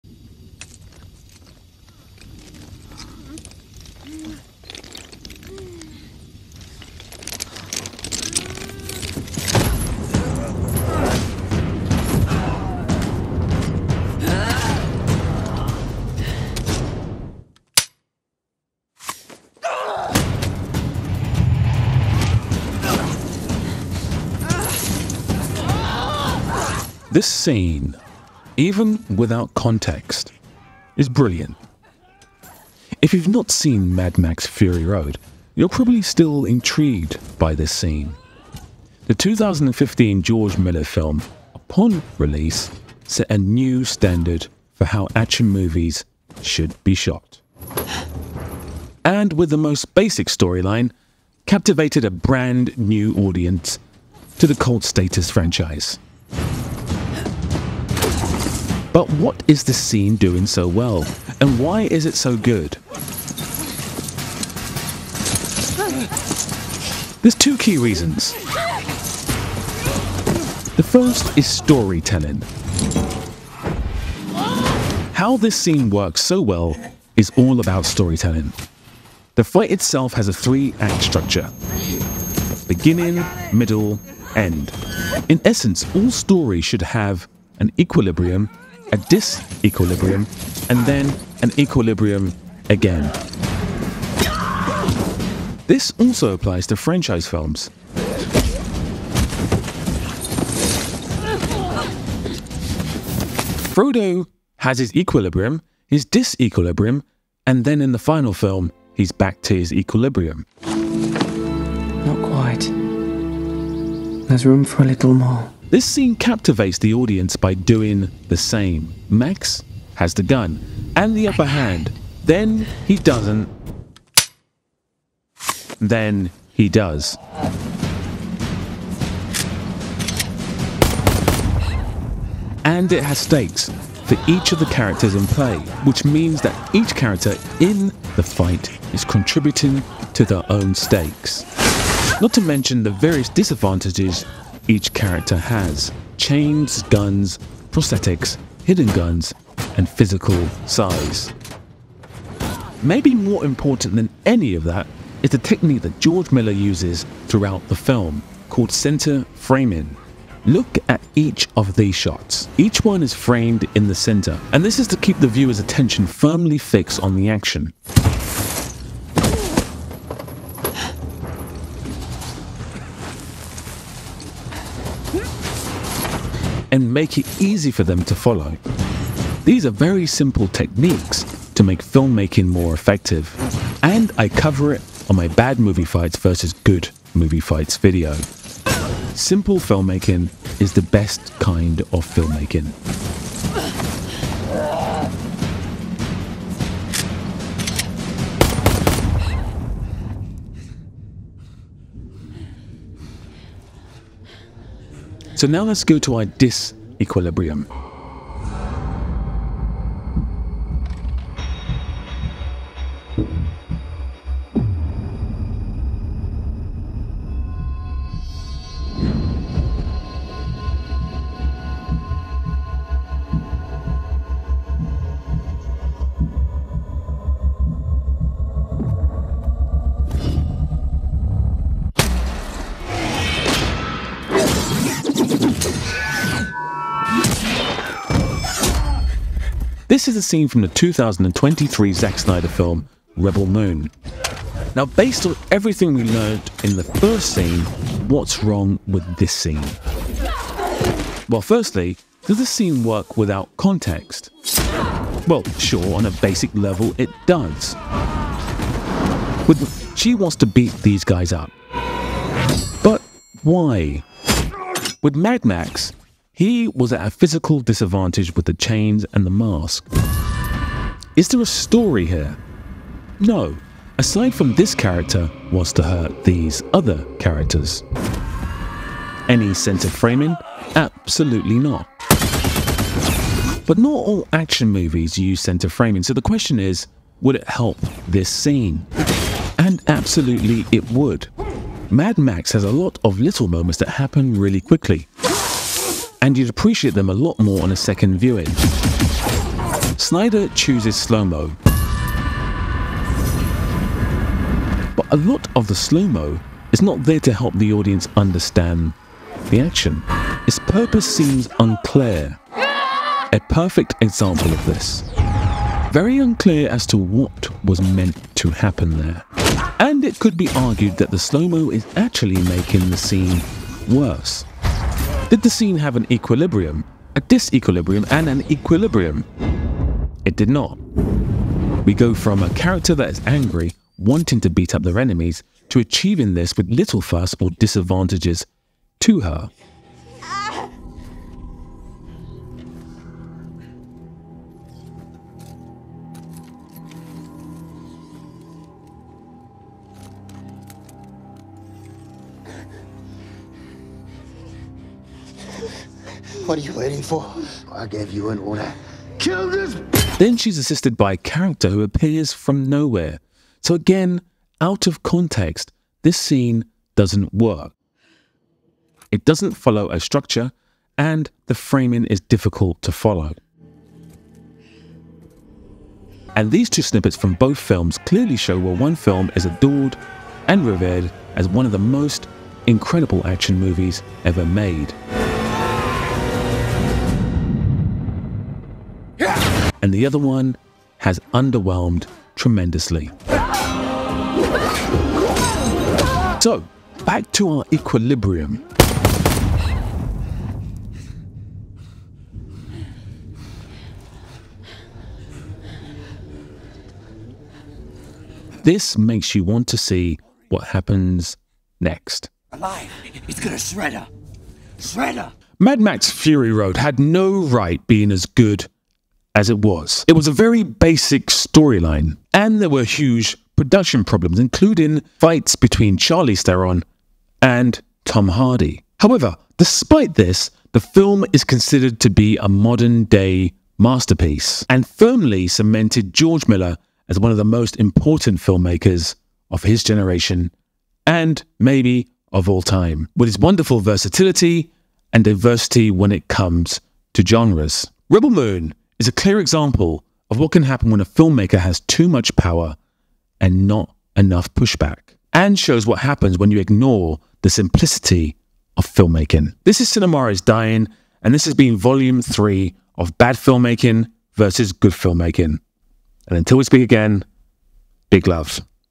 Bad Filmmaking vs Good Filmmaking Vol 3
With Furiso: A Mad Max story hitting theaters let's analyse Mad Mad: Fury Road and work out why it's filmmaking far eclipses that of Zack Synder's Rebel Moon.